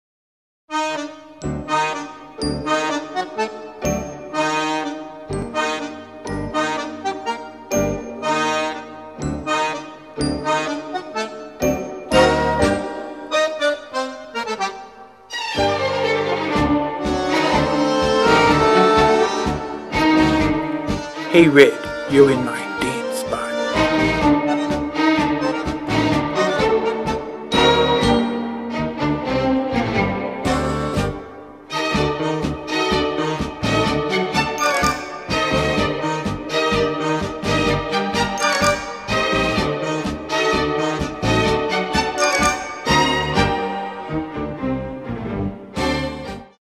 Hey Red, you're in my spot!
Twenty-Eight the cat has a few problems with Red the Chihuahua invading his space. This video is dedicated to the memory of "Lilly" the Radford, Virginia cat.

Follow Muffy Loves Blaine too.